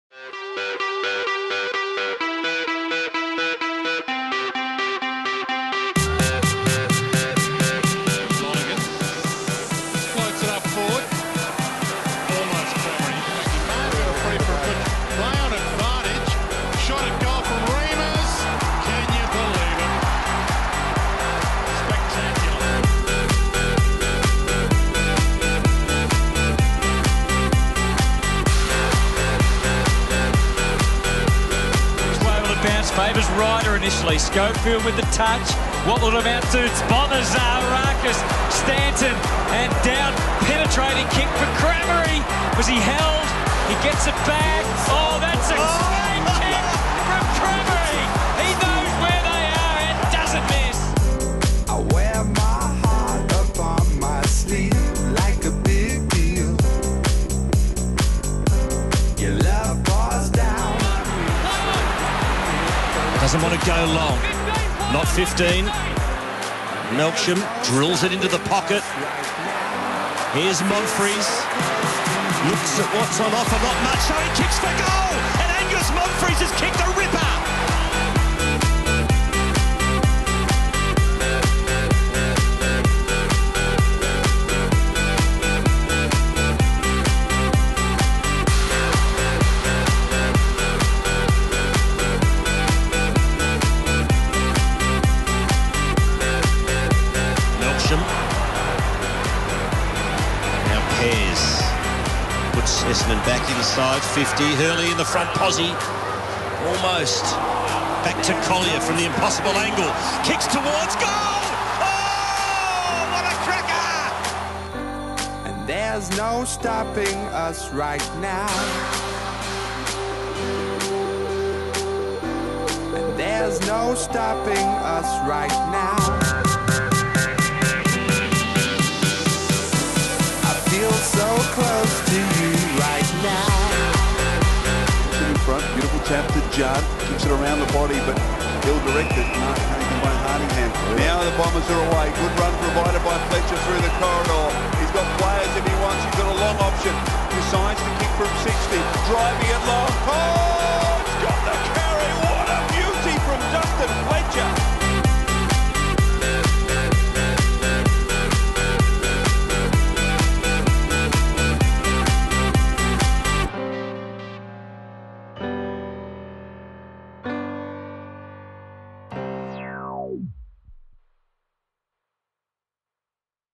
Essendon Bombers Goals of the Year 2011
Top 5 Goals of the Essendon FC of Season 2011